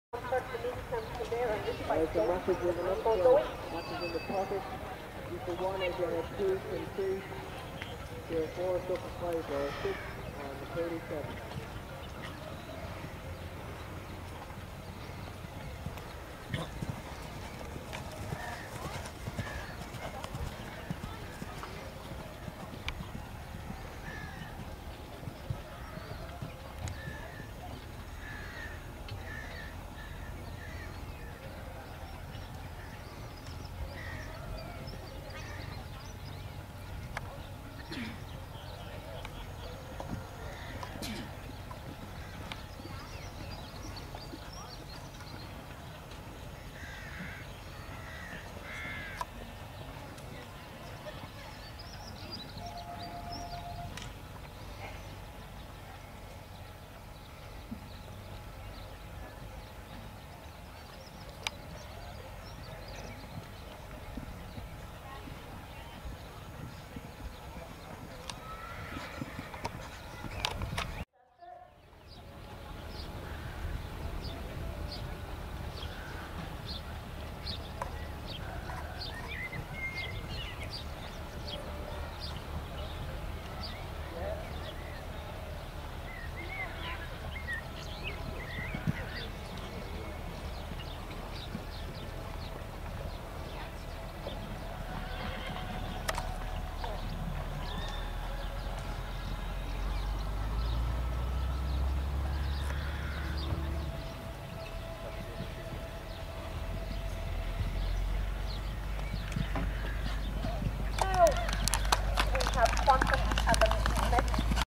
Gran Torino Barnadown Starfinder
Gran Torino 7yr old 148cm gelding by Puissance. Barnadown Starfinder Qual 27/05/12 finishing 2nd place :)